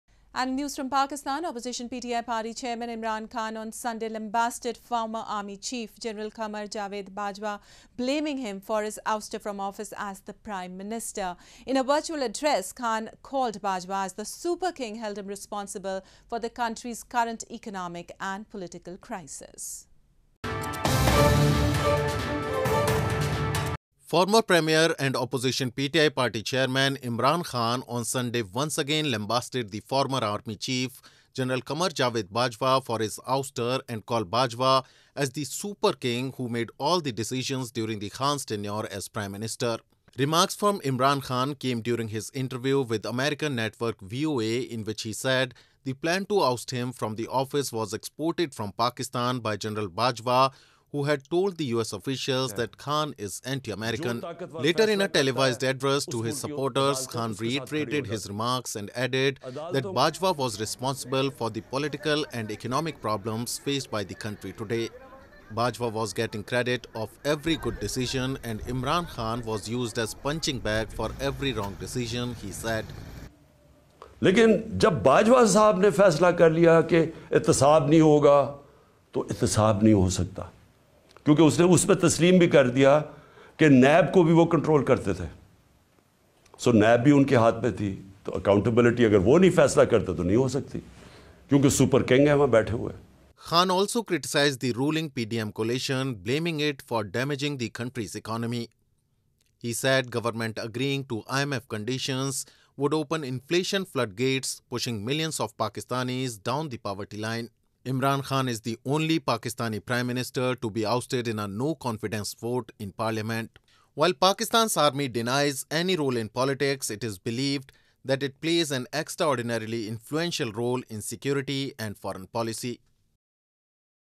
South Asia Newsline (date format without bracket - March 09, 2020) English News Bulletin
Opposition PTI party chairman Imran Khan on Sunday lambasted former army chief General Qamar Javed Bajwa, blaming him for his ouster from office as the prime minister. In a virtual address, Khan called Bajwa as the 'Super King' and held him responsible for the country's current economic and political crisis.